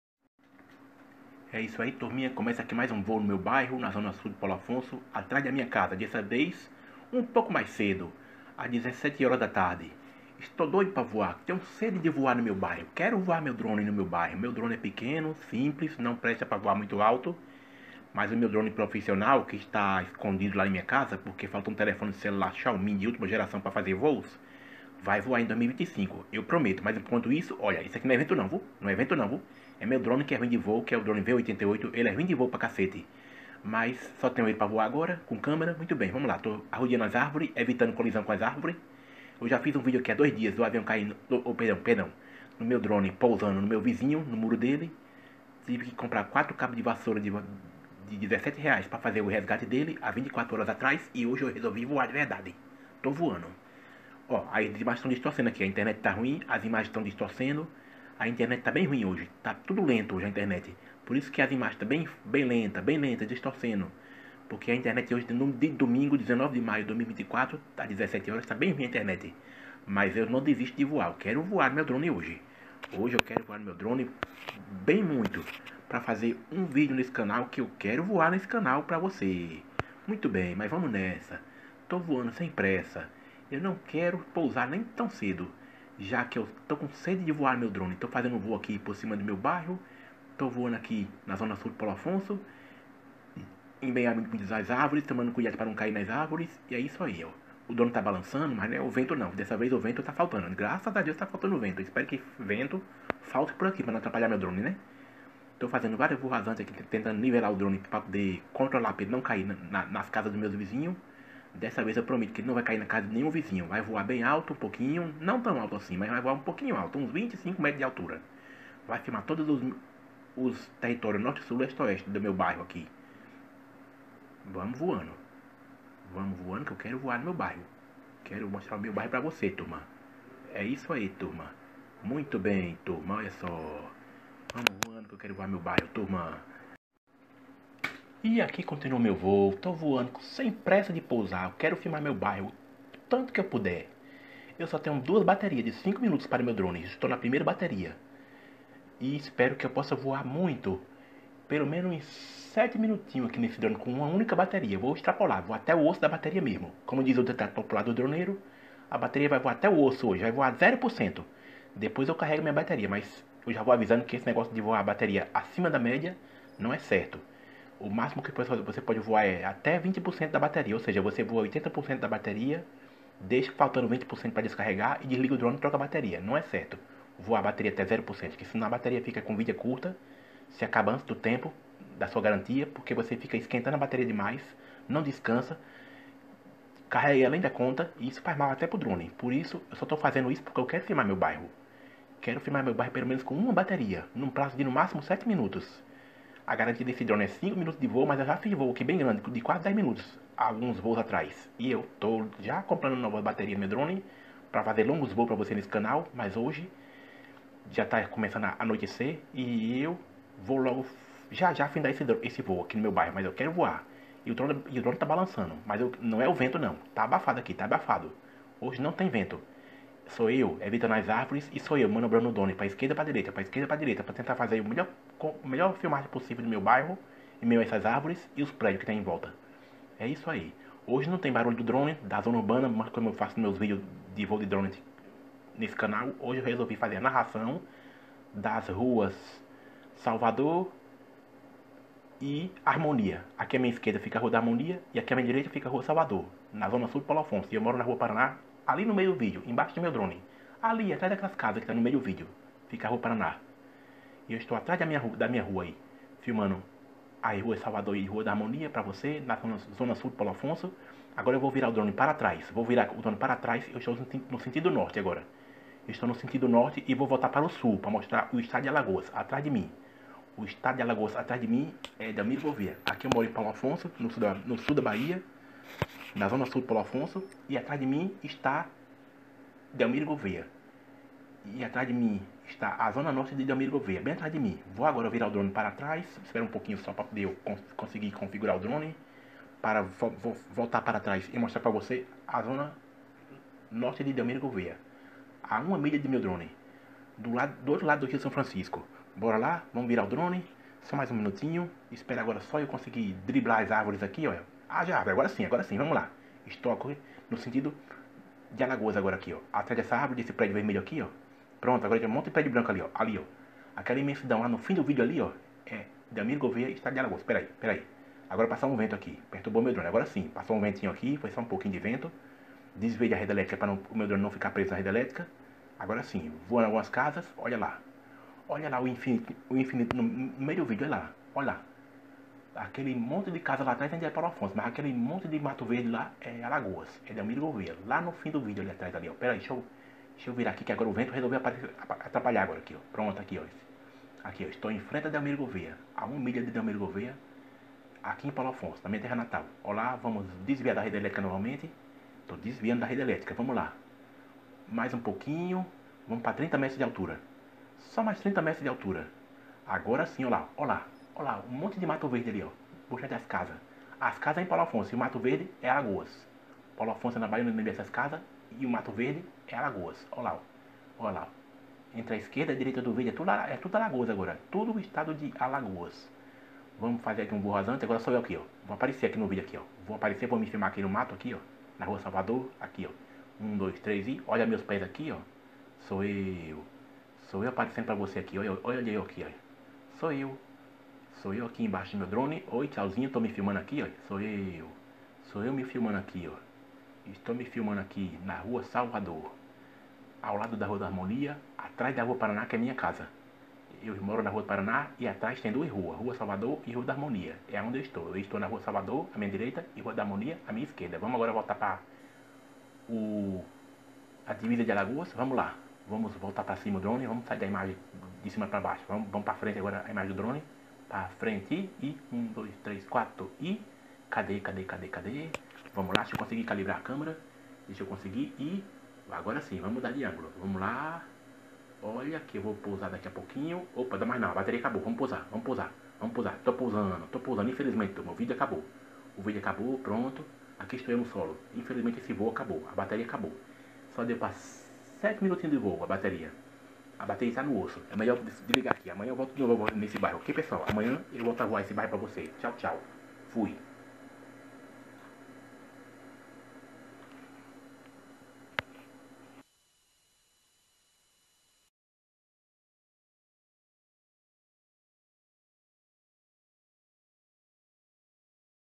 Mais um rápido sobrevoo no meu bairro com meu drone V88 as 17:00hs de 19/05/2023
ATENÇÃO... Estou também no telegran